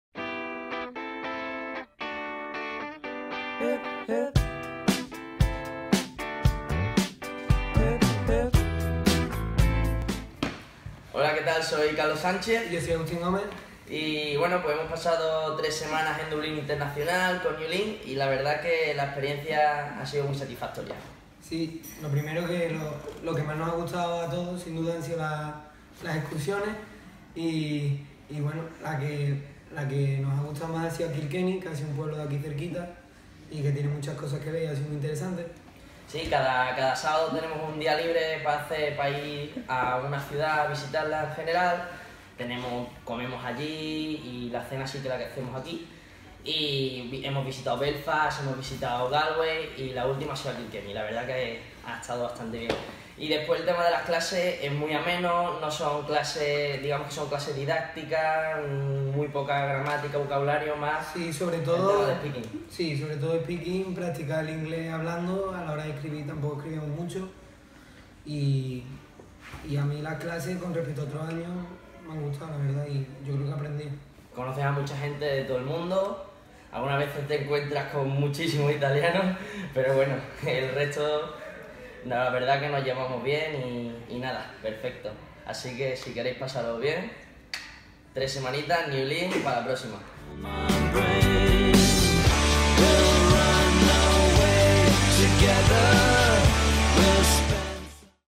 Newlink: Mi Verano en Dubín 2016 - Carlos Sánchez y Agustín Gómez
Carlos y Agustín nos cuentan su experiencia en el programa para jóvenes de Dublín Internacional en Irlanda.